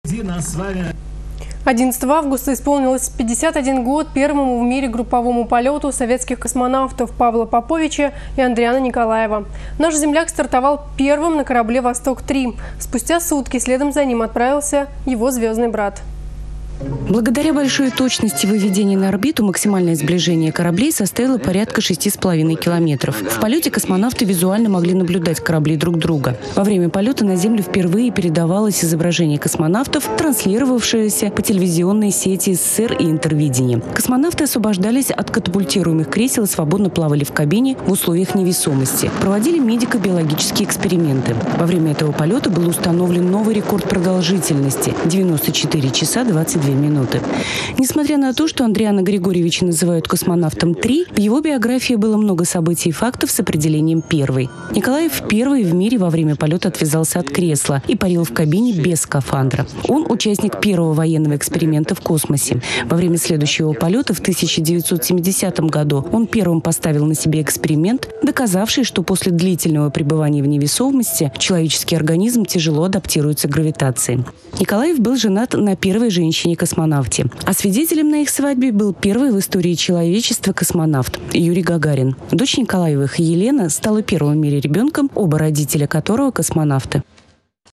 51 год первому в мире групповому полету советских космонавтов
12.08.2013
11 августа исполняется 51 год первому в мире групповому полету советских космонавтов — Павла Поповича и Андриана Николаева. Наш земляк стартовал первым на корабле Восток-3, спустя сутки следом за ним отправился его звездный брат.